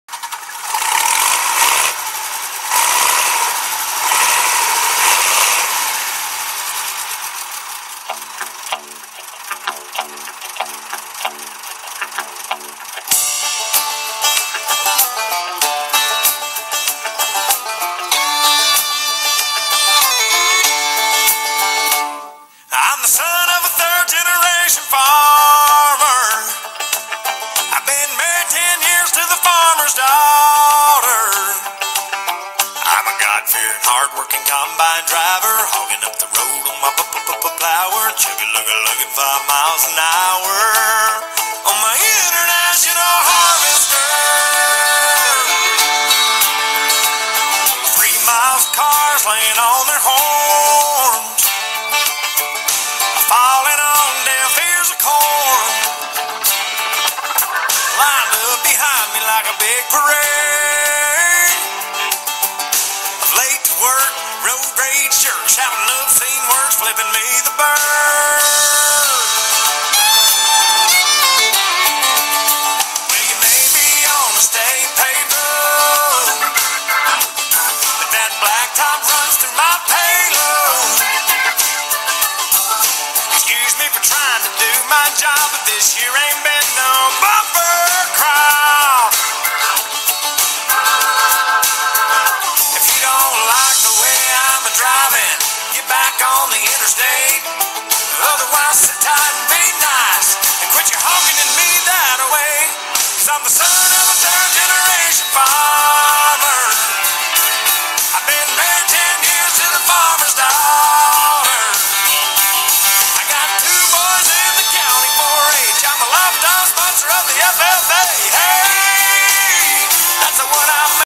pictures into video
Clay Wright pulling his Farmall C in Frenchville, Me July 7